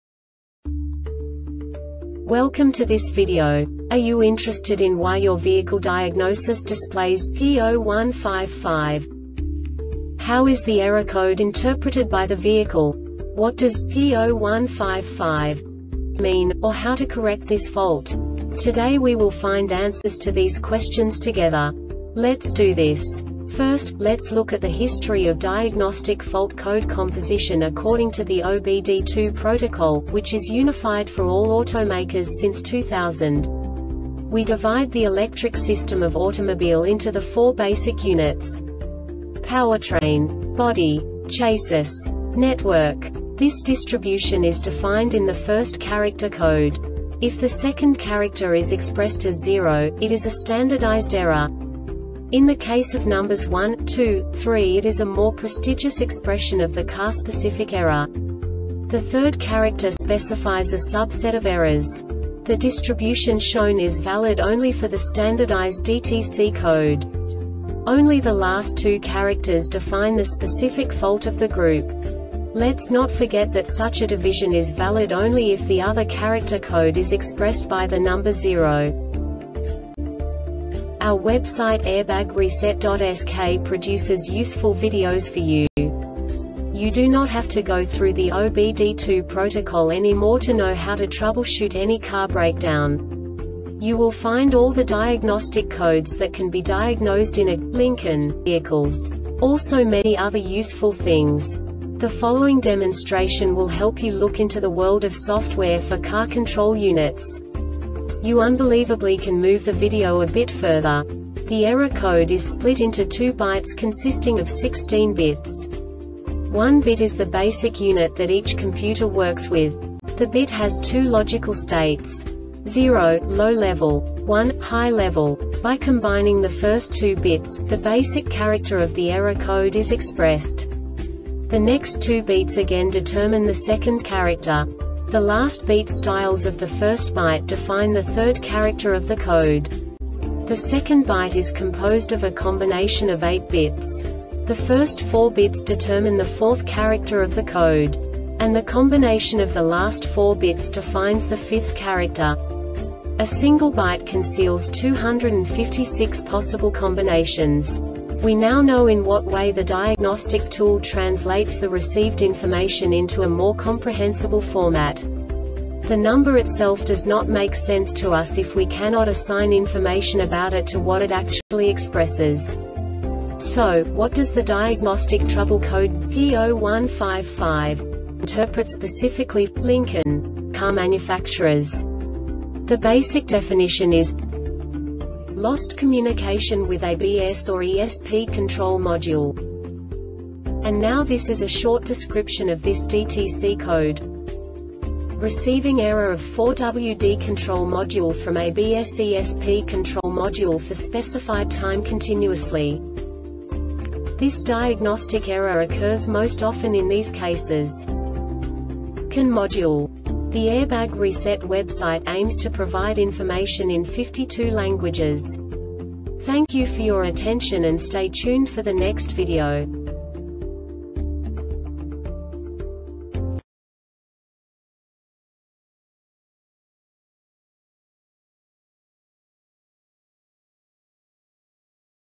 DTC Lincoln P0155 Short Explanation
Contents:
0:21 Basic DTC analysis according to OBD2 protocol standard.
1:48 Insight into programming
2:58 A diagnostically understandable description of the Diagnostic Trouble Code
3:05 Basic error definition

Make:
Lincoln
Code:
P0155
Definition:
Lost Communication With ABSorESP® Control Module
Description:
Receiving error of 4WD control module from ABSorESP control module for specified time continuously.
Cause:
CAN Module